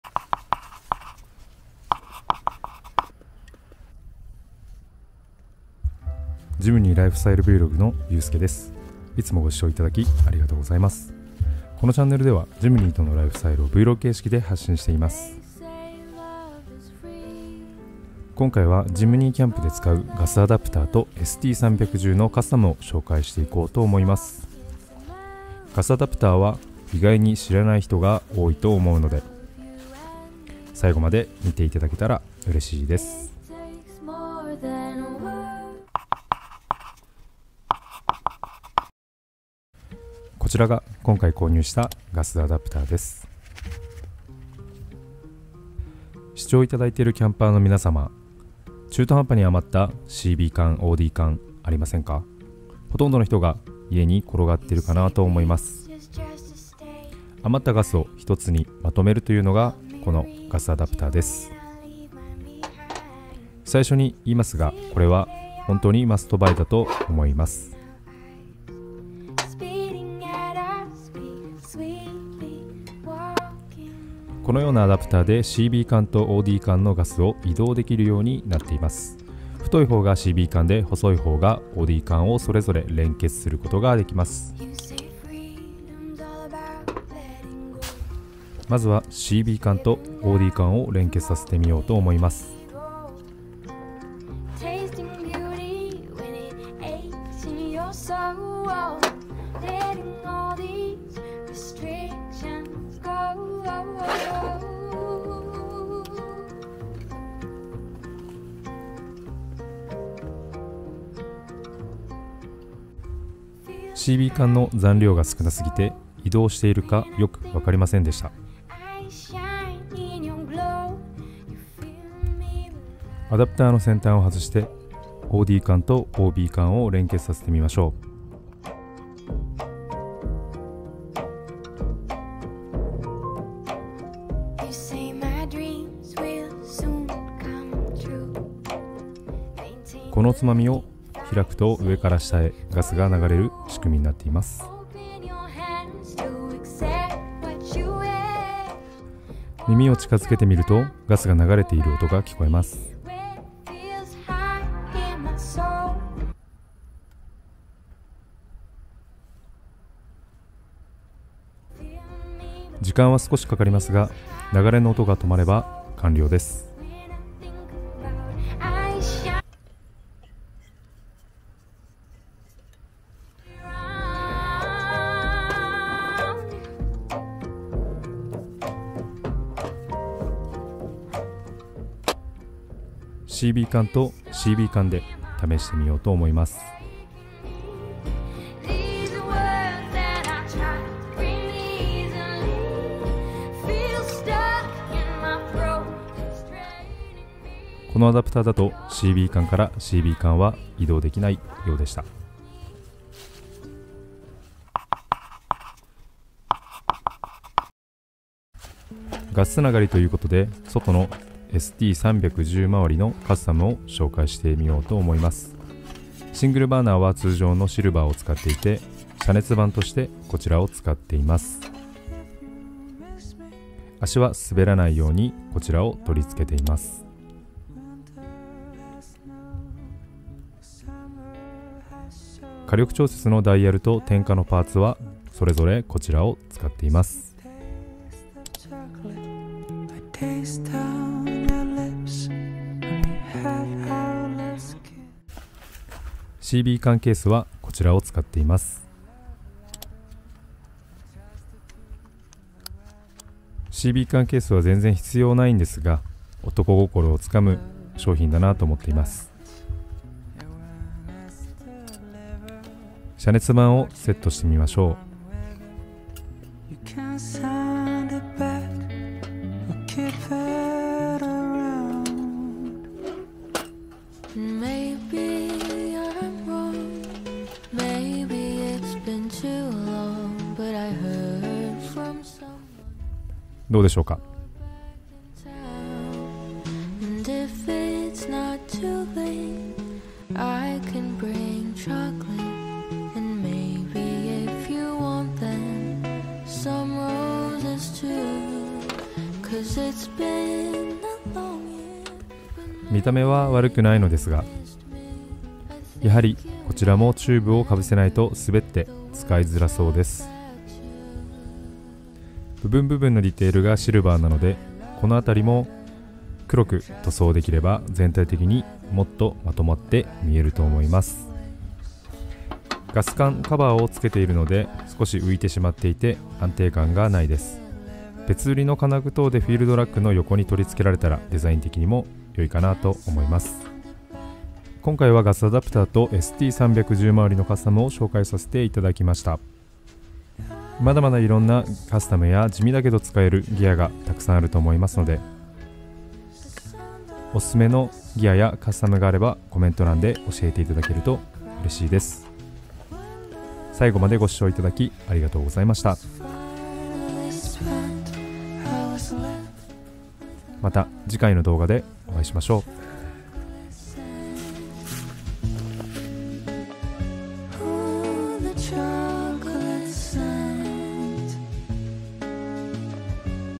【ジムニーソロキャンプ】知ってますか？地味に使えるガスアダプター。
Jimny Lifestyle Vlogでは、ジムニーとの生活をVlog形式で毎週発信していきます。
第65話としてキャンプで使えるガスアダプター動画となります。
使いかけのガスが残ってる方の購入検討している方の参考になれば嬉しいです。

【目次】
00:40  ガスアダプター紹介
03:29  シングルバーナーカスタム
06:07 おすすめ動画